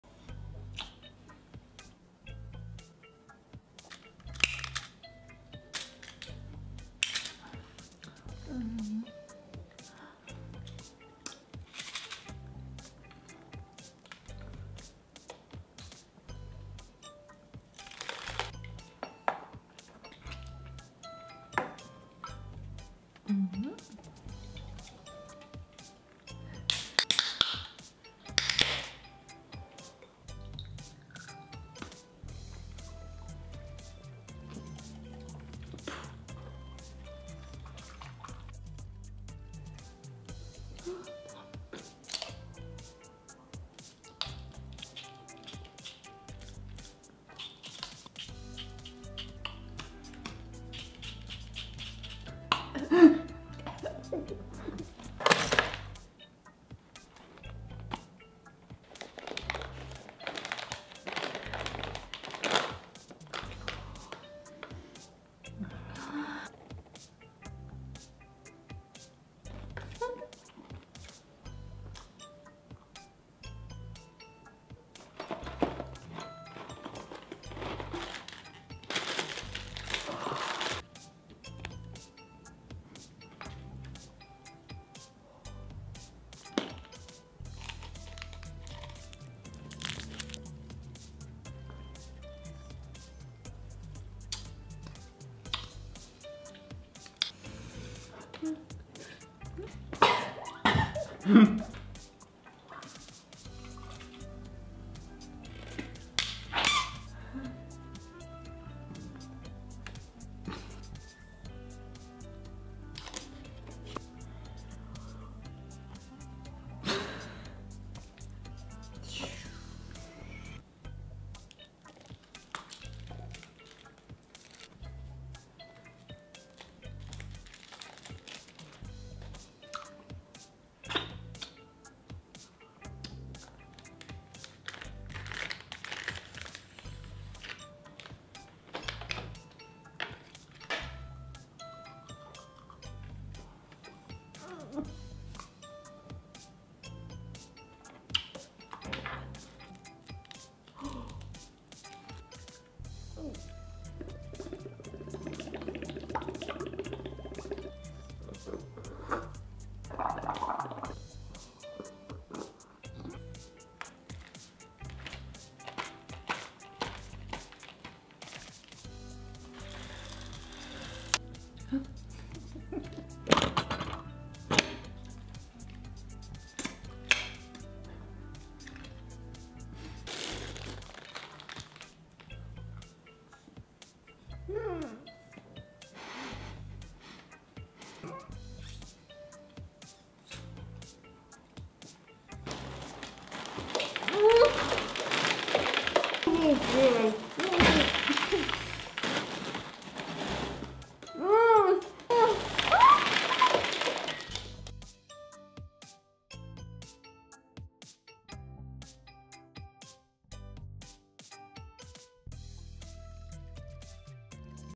Kırmızı Yemek VS Mavi Yemek Meydan okuma | ASMR Mukbang MARI ZD Türk
MARI ve arkadaş kırmızı yemek vs mavi yemek meydan okuma!  ASMR Mukbang MARI